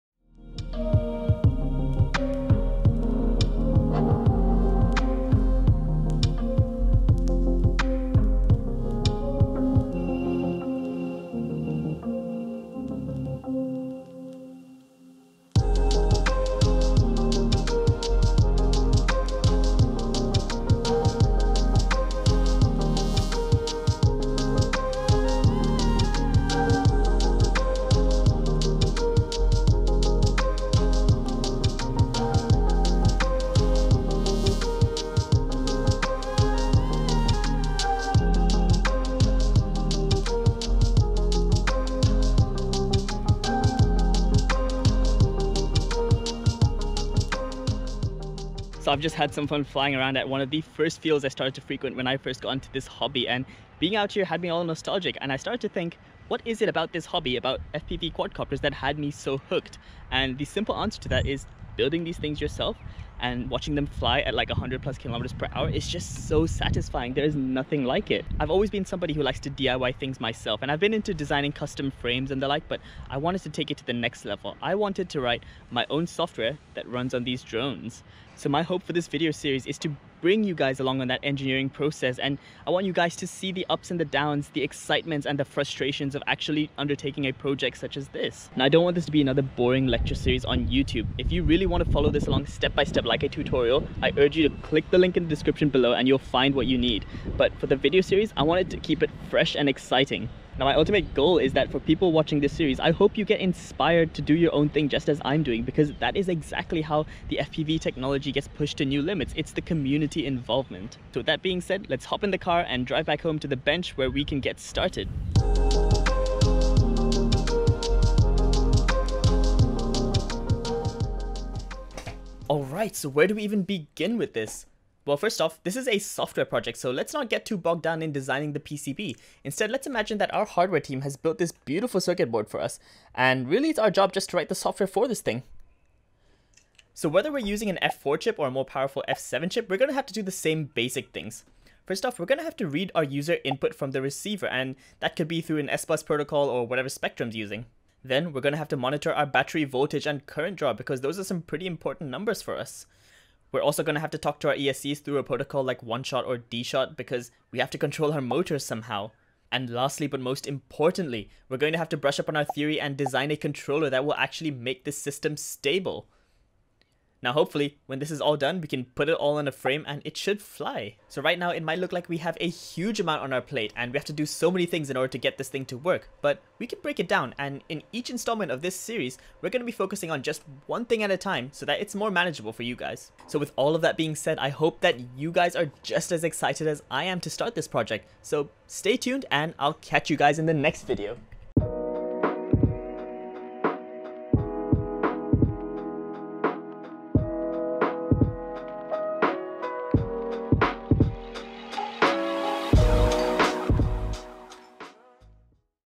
How to build your own flight controller | Part 1: motivation
In this video series, I'll take you along the process of designing the software for a quadcopter flight controller from scratch.